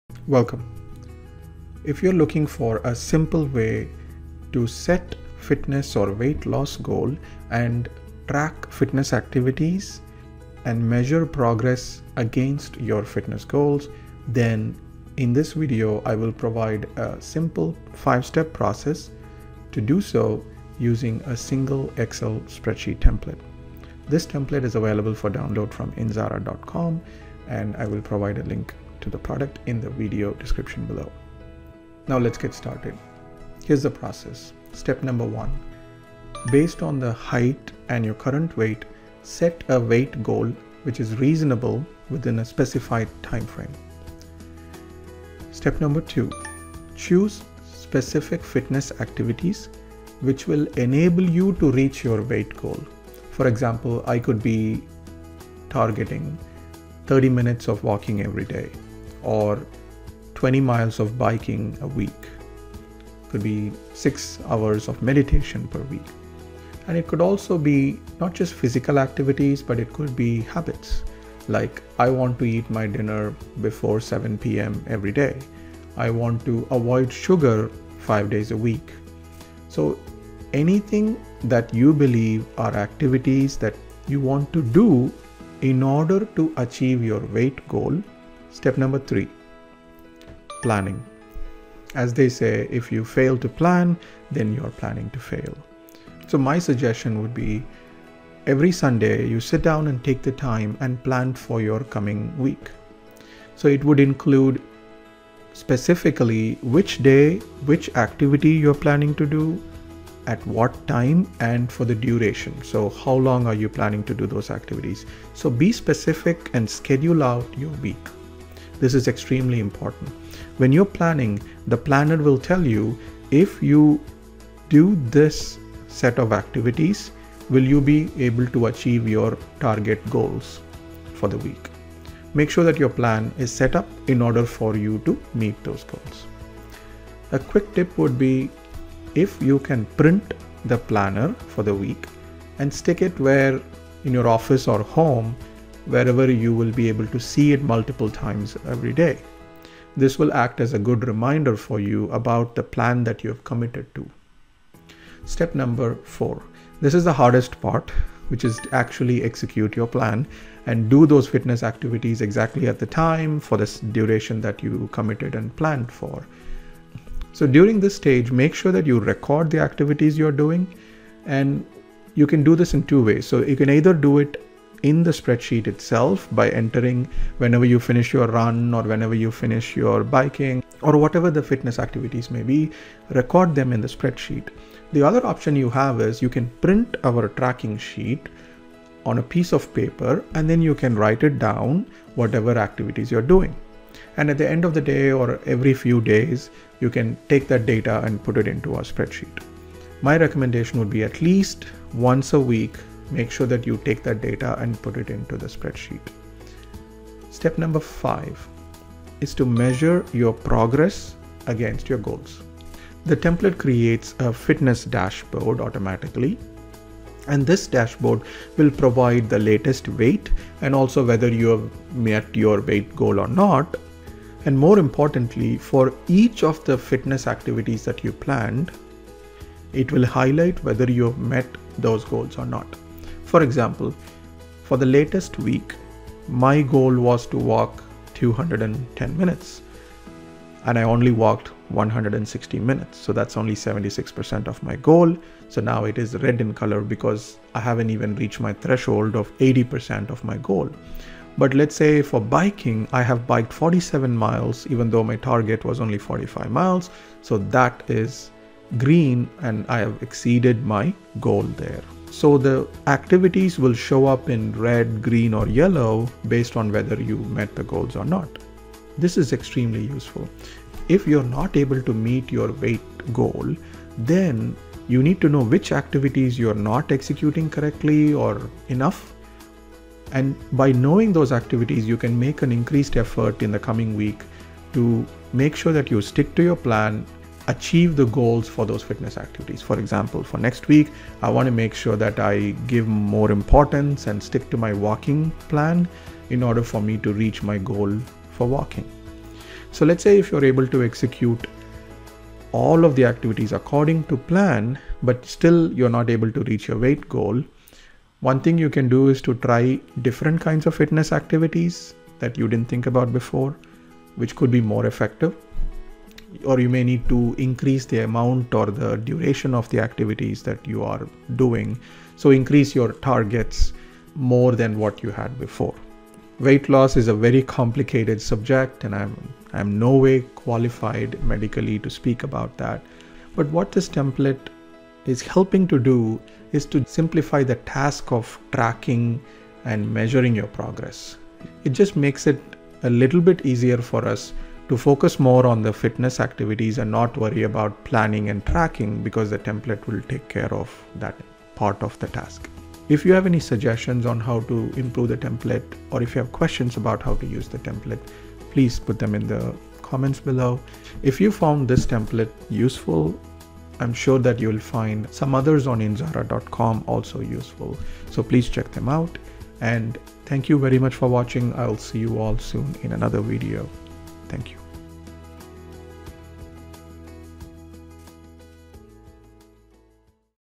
How to plan and track weight loss using an Excel Spreadsheet?
I present a simple and effective 5-step process to define your fitness or weight loss goal and track your progress using a simple Excel spreadsheet.

You can download this Weight Loss Tracker Excel template from indzara.com.

Here is the 5 step process
1. SET WEIGHT GOAL Enter your basic information such as height and weight. Set a target weight goal. The template can handle both English and Metric system of measurements.

2. CHOOSE ACTIVITIES Choose specific fitness activities and define targets for each of those activities. For example, here I have 30 mins of walking per day, 20 miles of biking per week, meditate for 6 hours per week and so on. The activities can also be habits. For example, eat dinner before 7 pm 5 days a week. Do not eat sugar 4 days a week. So on.

3. PLANNING As they say, if you fail to plan, you are planning to fail.  Since each week could be a different schedule, take the time to plan your week. Every week, on Sunday, plan your activities for the coming week. Ensure that your plan will result in meeting your targets for each of the fitness activities. For example, if you target biking 20 miles per week, make sure that you schedule that in your plan.
a. Tip: Print the weekly plan and post it in your home or office where you will see it multiple times a day. Once you have a solid plan, next step is to execute your plan.

4. RECORD ACTIVITIES: Enter actual progress of activities daily. As you walk, bike, run or perform the activities planned, enter them in the Excel file. Also, record your weight.
a. Tip: Record progress on the printed paper daily and then enter in the Excel file weekly or when you have time.

5. MEASURE PROGRESS: The template automatically prepares the Fitness dashboard.
a. The Fitness Dashboard shows the latest weight and BMI and whether you have achieved your weight goal or not.
b. Each of the fitness activities are displayed with % of goal achieved. It will be green if goal is achieved, red if below the threshold and yellow if in between.
c. For example, 1132 mins of walking were completed out of a target of 840 mins. That is 135% of goal. Whereas in Biking, 121 miles were achieved out of a goal of 180 miles. That is only 67%.
d. If you are not achieving your weight goal, take the time to identify activities which you are not executing as planned. Modify your schedule and make a concerted effort to stick to your plan.
e. Please make a habit to enter data at least once a week and review the progress. This will help you make the necessary changes in habits to improve in upcoming week.